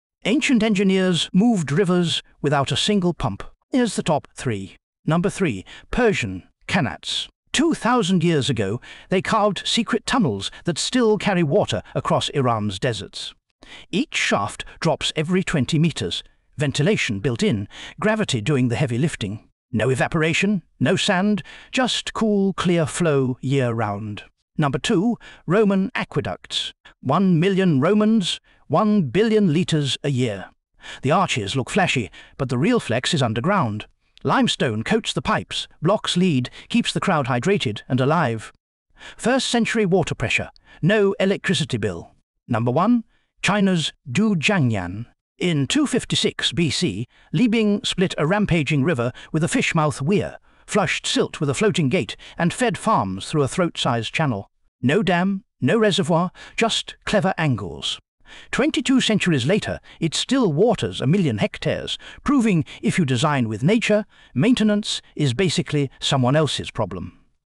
Top 3 Ancient Water Management Systems That Still Amaze Today!
Explore the most impressive ancient water management systems, including Persian Qanats, Roman aqueducts, and China's Dujiangyan. Discover how ancient engineers successfully harnessed and managed water resources without modern technology, and learn about their lasting impact on sustainable water management practices. This video reveals the fascinating history and engineering behind these incredible feats, highlighting their continued relevance in modern times.